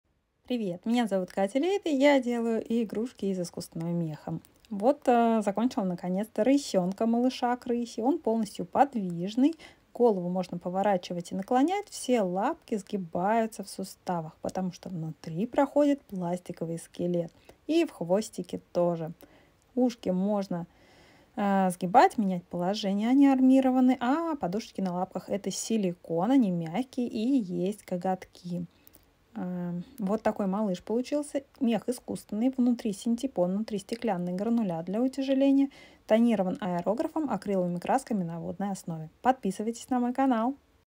Рысёнок из искусственного меха
Рысёнок сделан из искусственного меха. Полностью подвижный - внутри пластиковый скелет. Голову можно поворачивать и наклонять, лапки и хвост сгибать.
Ушки сгибаются. Подушечки на лапках - силикон. Коготки - запекаемая пластика.
Тонирован аэрографом акриловыми красками на водной основе.